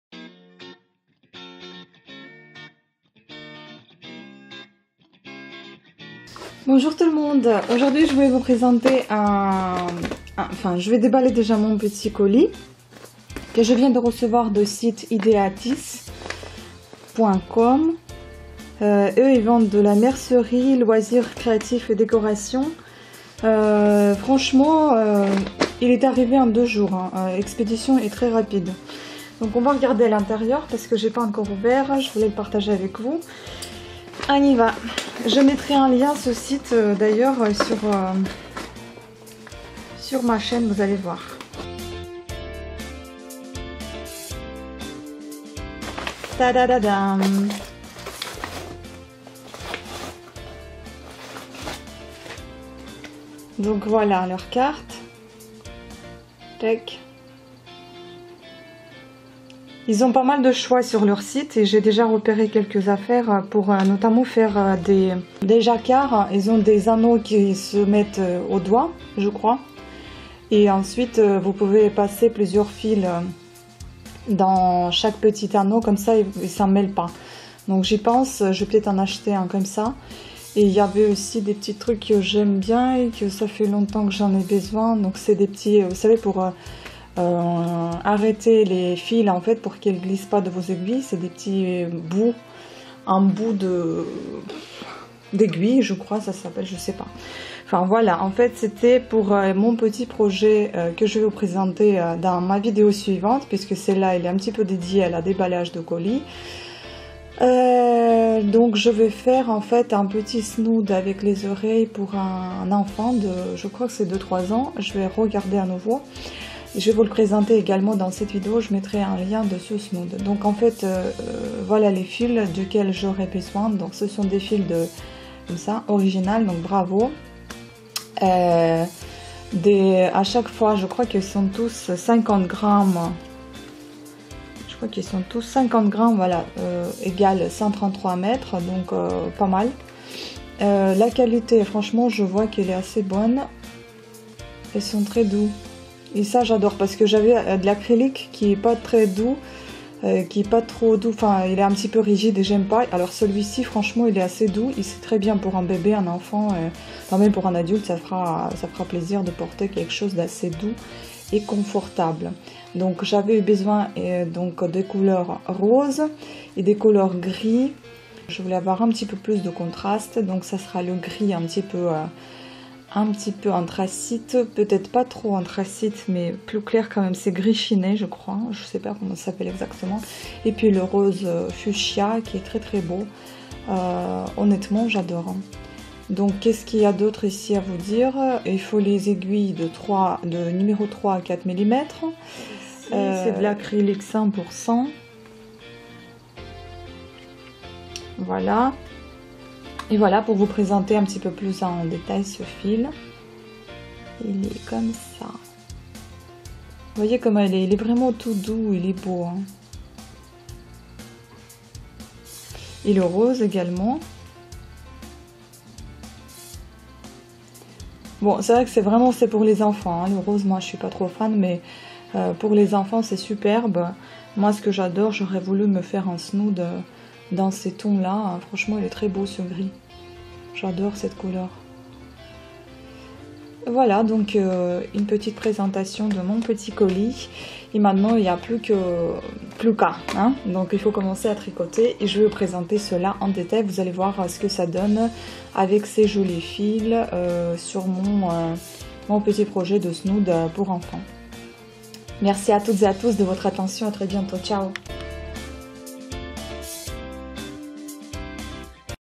Déballage colis laine Ideatiss/Unboxing knit #3
La vidéo présente le déballage d'un petit colis reçu de chez Ideatiss. La laine à tricoter de couleur rose et gris pour mon nouveau projet de snood avec les oreilles, snood souris , à suivre dans les prochaines vidéos. Faites un tour sur ma chaîne et vous pouvez découvrir mes ouvrages tricotés ainsi que la laine et les accessoires avec des noms de boutiques en ligne d'où ils proviennent.

Bonne visite à toutes et à tous!